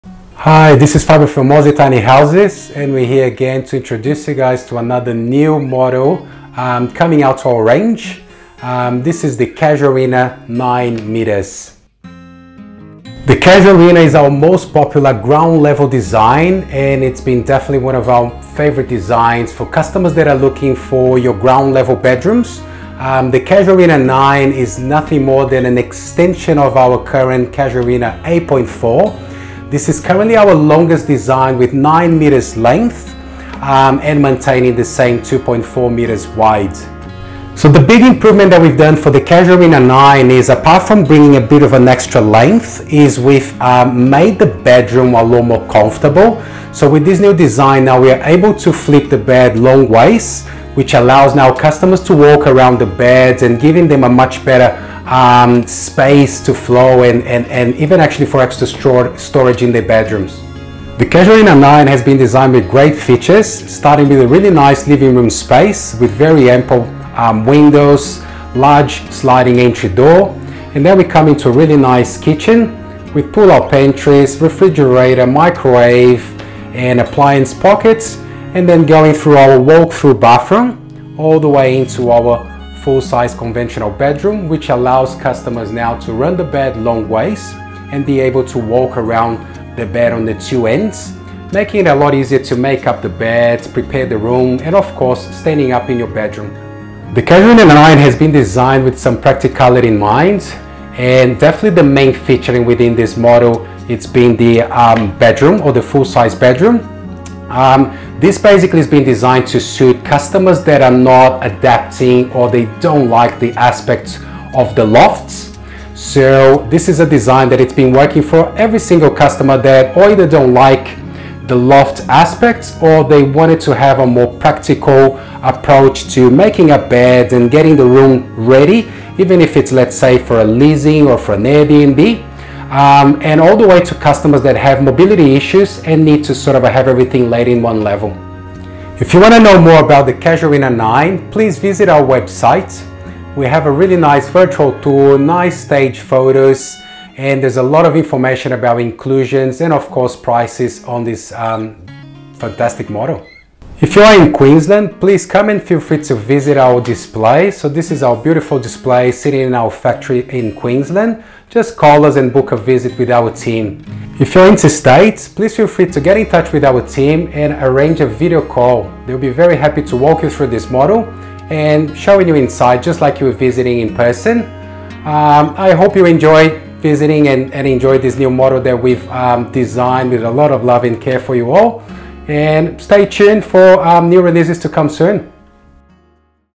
Casuarina 9.0 - Our Longest Tiny House Design With Separate Ground Level Bedroom and Storage Loft
The Casuarina 9.0 is our longest design available, built on a 9-meter trailer.

Complete with a large enclosed ground-level bedroom, double built-in wardrobes, a central bathroom and a spacious Kitchen and Living area.

The internal bathroom design creates an ensuite to the bedroom, and can also be accessed via the Kitchen and Living area.

The Kitchen provides adequate bench space with a full-height pull-out pantry, storage cupboard and space for a full-height fridge.

The 1.5-meter wide bathroom with shower, vanity, toilet and washing machine insert, is surprisingly large with plenty of space for storage and shelving options. There is also a storage loft above the bathroom for suitcases, boxes and other miscellaneous items.

This Tiny House is truly one to enjoy, a spacious and well-appointed “Home” to be proud of.